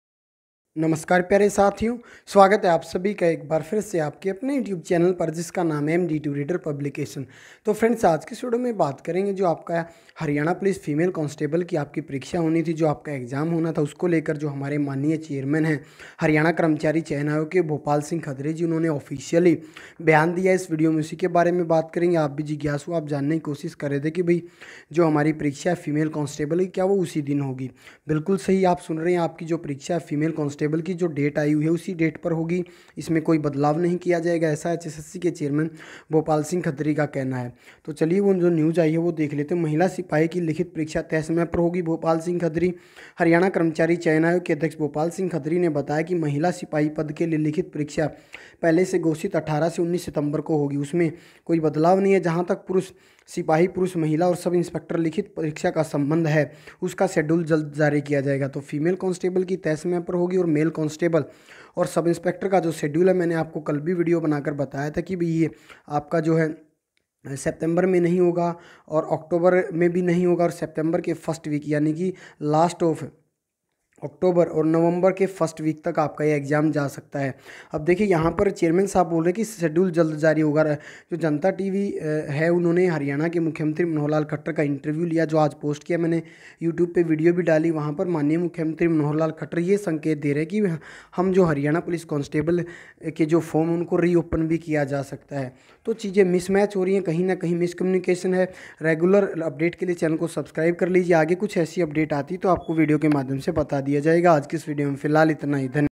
Haryana Police Female Constable Exam तय समय पर होगा : भोपाल सिंह खदरी
Haryana Police Female Constable Exam Date News

Hp Female Constable Exam Date News